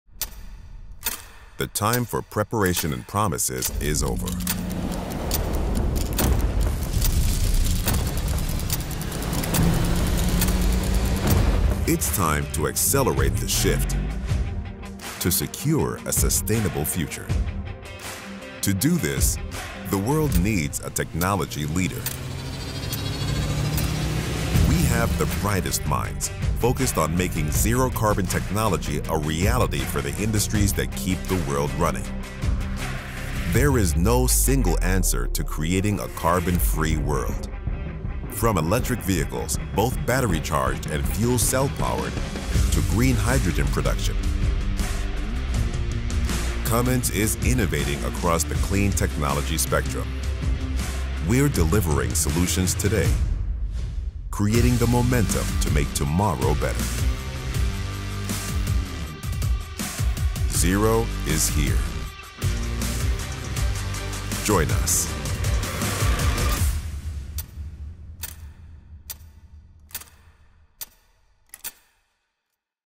Zero is Here
As the threat of climate change increases, New Power continues to innovate and accelerate the adoption of zero-carbon technology. With more than 2,000 fuel cells, 600 electrolyzers and 500 battery electric powertrains deployed globally, one thing is clear:

We're already at zero.
Join us.